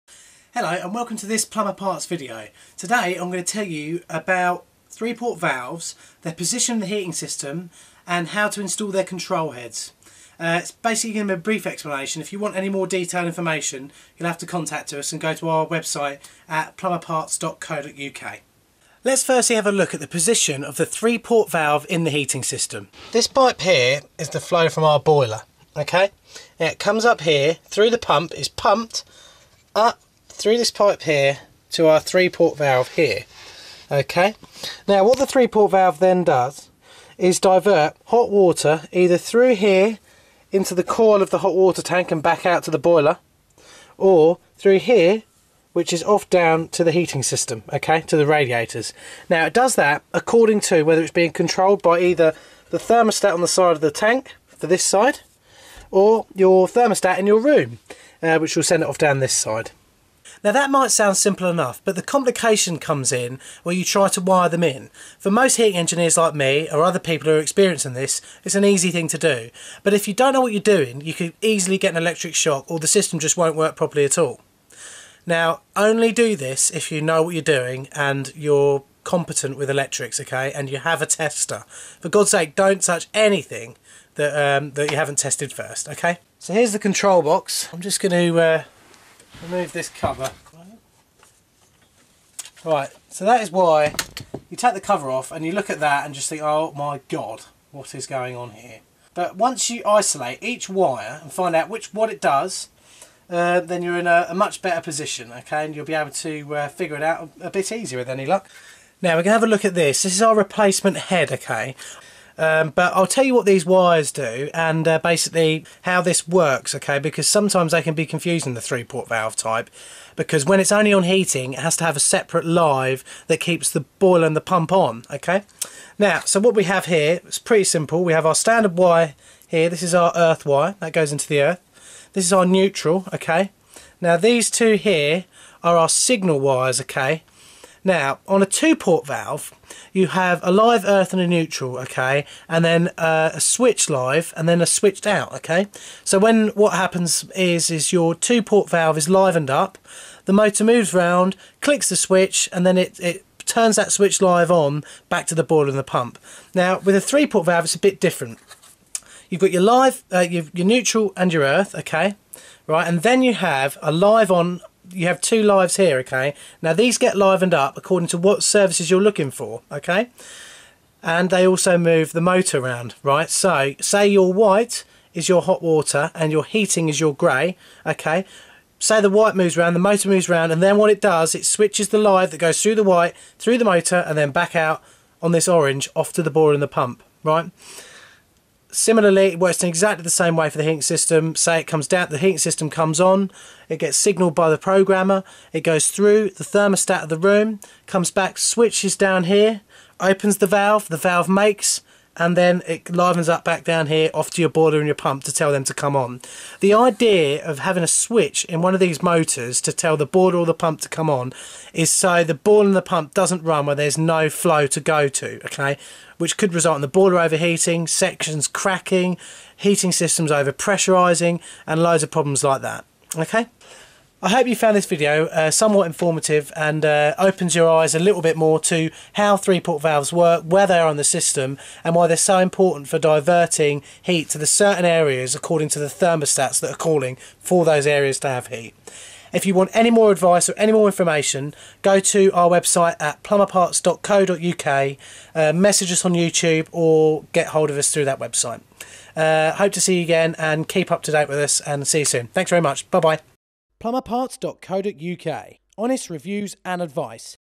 3 Port Valves and Y Plan Heating Systems - Plumbing Tips
This video covers the 3 port valve. In particular it's position in a Y plan system. We also cover the basics of installing or replacing a three port valve or head.